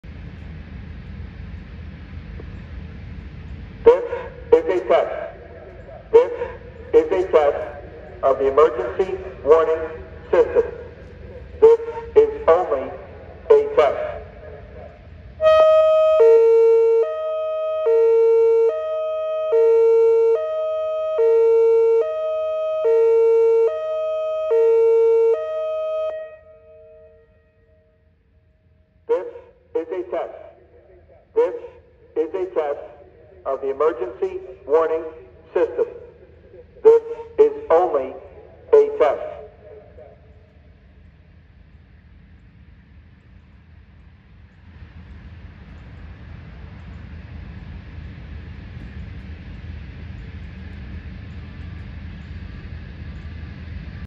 Whelen WPS-2902, Siren Test, Short Hi-Lo, Franklin, VA
Filmed 11/4/2020

Siren Info:
Siren Name: Whelen WPS-2902
Active: Yes
Replaced: Nothing
Installed: 2013
Condition: Excellent
Uses: Plant Warnings
Sound Source: Speaker
Tone Type: Single Tone
Port Ratio: None
Siren Controller: Whelen ESC-2030
Mount: Wooden Pole
Duration: 12 seconds of Voice, 10 seconds of Hi Lo, 12 seconds of Voice
Testing: Every Wednesday at 9:00 AM EST
Time When Siren Activated: 9:00 AM (Siren sounded just in time)
Siren Loudness: 108.5 Decibels (Very Loud)
Other Units Heard: None
Scanner Used: None
Other Enthusiasts Present: None
What cancels the test:
1. Problem sending the signal to the siren.